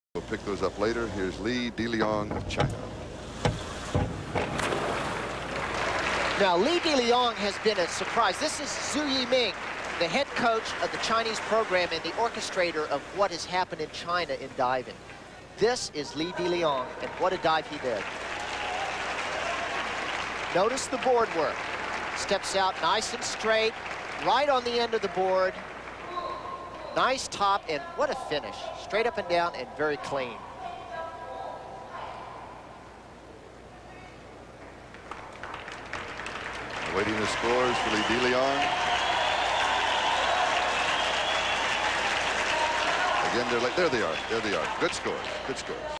1988 Deliang Li CHN - 103b - 8.5s - 3 Meter - Olympics
1988 Seoul Olympics, Deliang Li from China in the Men's 3m finals doing a 103b for 8.5s. He will take home a bronze medal in this event